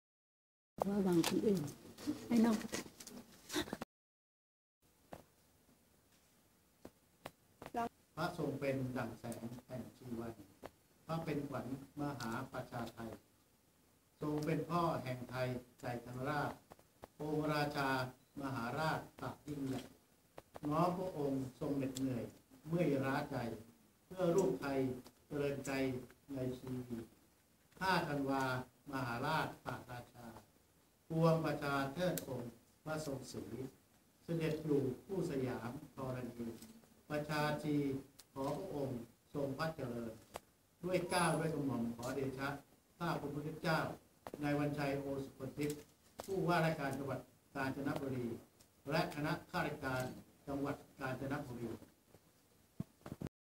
ผวจ กาญจนบุรี บันทึกเทปถวายพระพร
ผู้ว่าราชการจังหวัดกาญจนบุรี คณะหัวหน้าส่วนราชการ และเหล่ากาชาดจังหวัดกาญจนบุรี บันทึกเทปโทรทัศน์ถวายพระพรเนื่องในโอกาสมหามงคลเฉลิมพระชนมพรรษาพระบาท สมเด็จพระเจ้าอยู่หัวฯ 5 ธันวาคม 2557 พร้อมทั้งมอบนโยบายในการทำงานให้กับข้าราชการสำนักประชาสัมพันธ์เขต 8 กาญจนบุรี